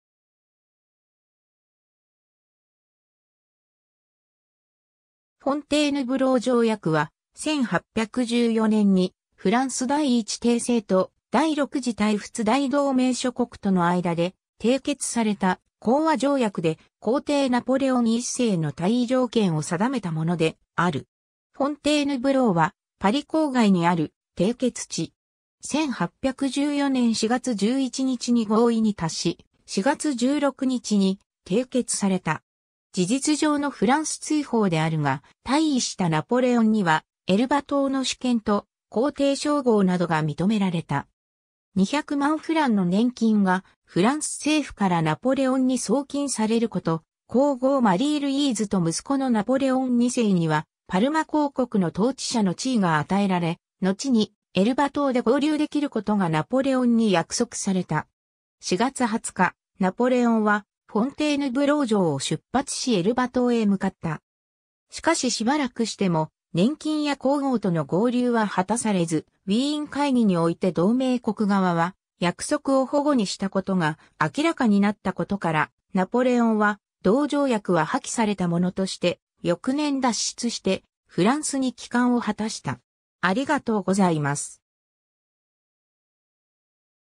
フォンテーヌブロー条約 (1814年)
フォンテーヌブロー条約（フォンテーヌブローじょうやく、仏: Traité de Fontainebleau, 英: Treaty of Fontainebleau, 露: Договор в Фонтенбло, 独: Vertrag von Fontainebleau）は、1814年にフランス第一帝政と第六次対仏大同盟諸国との間で締結された講和条約で、皇帝ナポレオン1世の退位条件を定めたものである。
 フォンテーヌブローは、パリ郊外にある締結地。
 1814年4月11日に合意に達し、4月16日に締結された。
事実上のフランス追放であるが、退位したナポレオンにはエルバ島の主権と皇帝称号などが認められた。
 200万フランの年金がフランス政府からナポレオンに送金されること、皇后マリー・ルイーズと息子のナポレオン2世にはパルマ公国の統治者の地位が与えられ、のちにエルバ島で合流できることがナポレオンに約束された。
 4月20日、ナポレオンはフォンテーヌブロー城を出発しエルバ島へ向かった。
しかししばらくしても年金や皇后との合流は果たされず、ウィーン会議において同盟国側は約束を反故にしたことが明らかになったことから、ナポレオンは同条約は破棄されたものとして翌年脱出して、フランスに帰還を果たした。